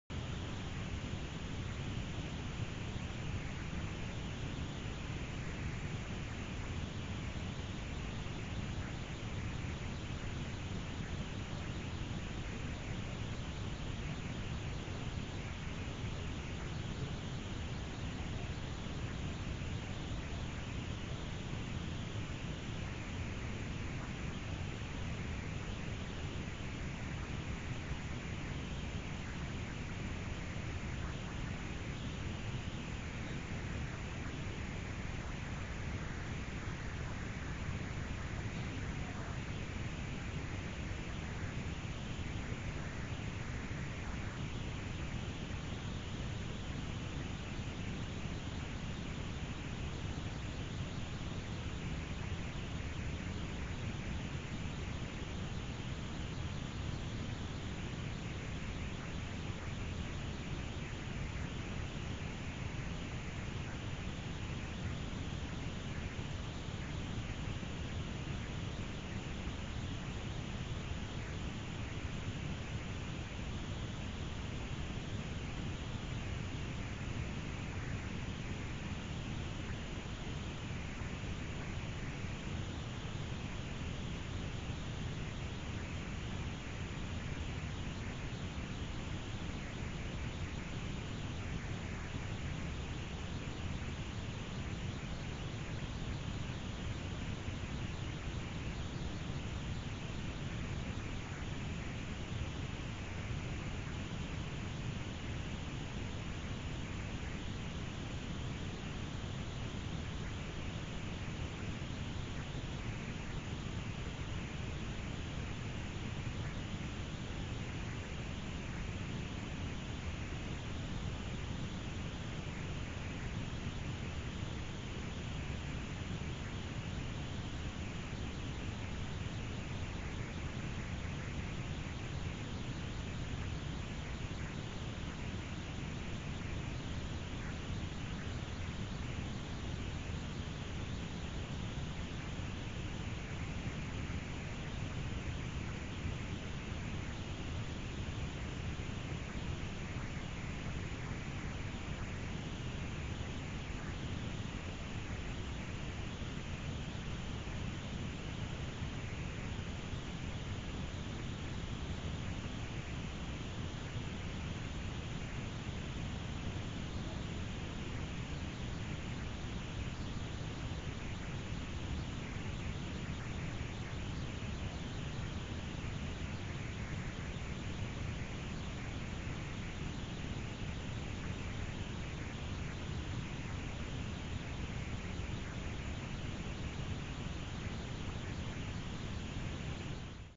paranormal activity? poltergeist? (23 seconds into video the bike reflector moves!!! )
possible poltergeist activity caught on camera.